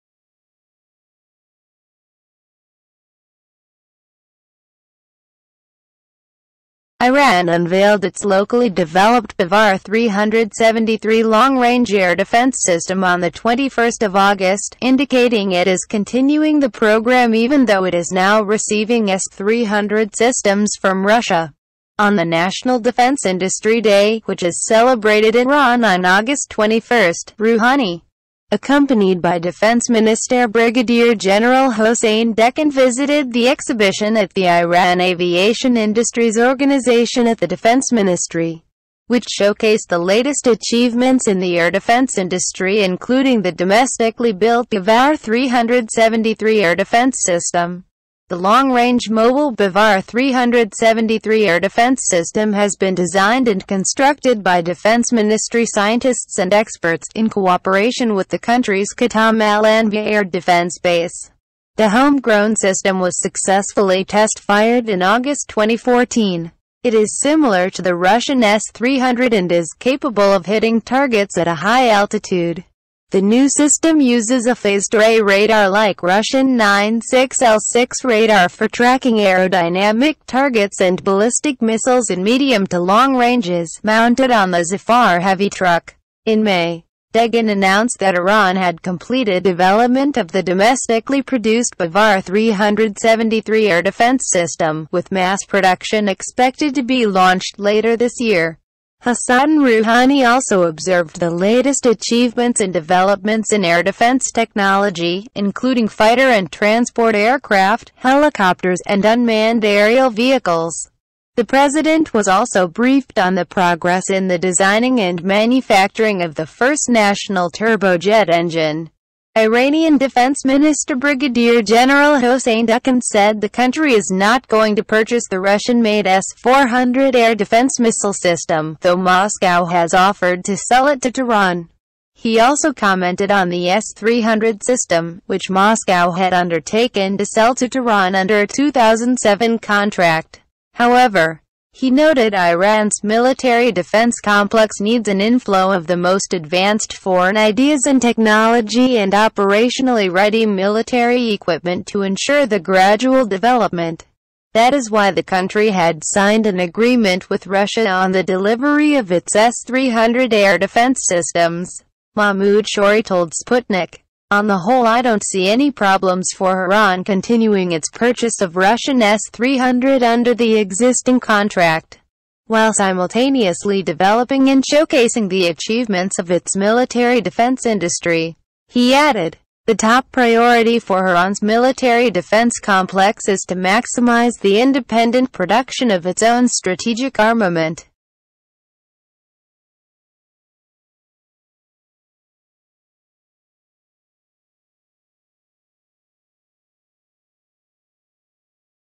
Iran unveils Bavar-373 long-range mobile air defense system
Iran unveiled its locally developed Bavar-373 long-range air-defence system on 21 August, indicating it is continuing the programme even though it is now receiving S-300 systems from Russia. Rouhani, accompanied by Defense Minister Brigadier General Hossein Dehqan, visited the exhibition at the Iran Aviation Industries Organization at the Defense Ministry, which showcased the latest achievements in the air defense industry including the domestically-built Bavar-373 air defense system.

The long-range mobile Bavar-373 air defense system has been designed and constructed by Defense Ministry scientists and experts in cooperation with the country’s Khatam al-Anbia Air Defense Base. The home-grown system was successfully test-fired in August 2014. It is similar to the Russian S-300 and is capable of hitting targets at a high altitude. The new system uses a phased array radar like Russian 96L6 radar for tracking aerodynamic targets and ballistic missiles in medium to long ranges, mounted on the ZAFAR heavy truck.